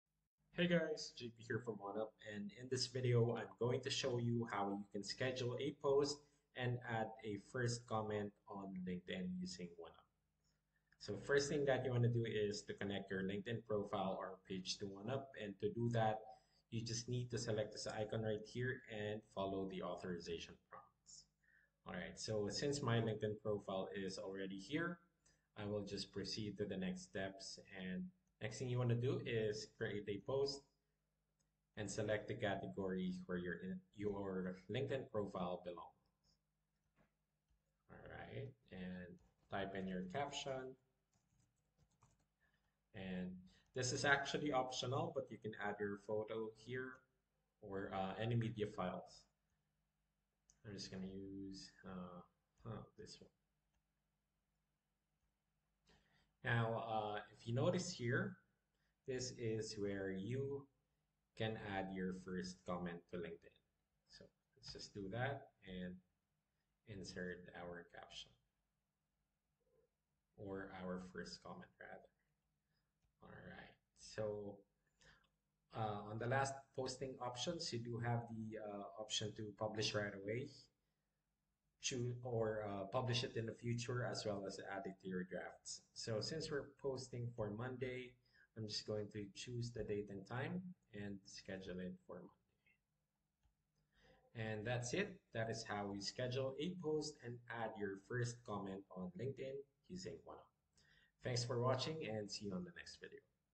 How to Schedule a LinkedIn Post including a First Comment
Did you know you can easily schedule your LinkedIn posts and add a first comment using OneUp? 🤓 It's super easy and convenient!